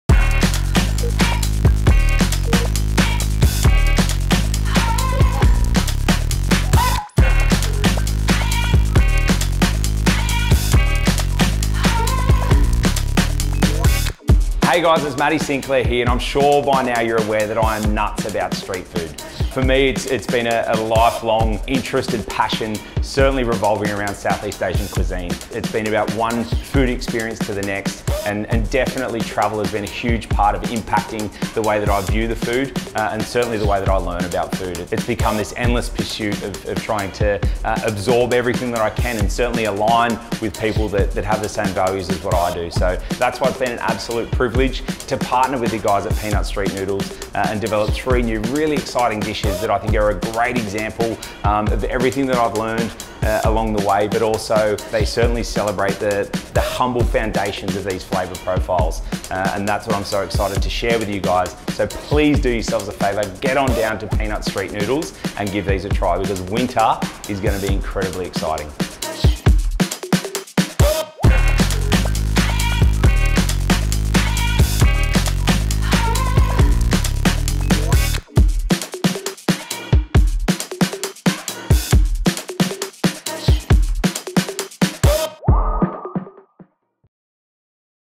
Matt Sinclair x P'Nut Street Noodles
We’ve partnered up with former MasterChef finalist and award-winning Chef Matt Sinclair to bring you three exclusive new dishes available for a limited time only at P'Nut Street Noodles!

Each dish will take you on a taste journey straight to the streets of Asia and bring alive the humble and delicious flavours of street food, one delicious slurp at a time.

Inspired by Matt’s modern and fun approach to classic Asian flavours, enjoy a twist on a classic with his Green Curry Fried Rice, rich and velvety Hoisin Duck Noodles, and an aromatic and warming Thai Coconut Soup available across all P’Nut locations in-store and online!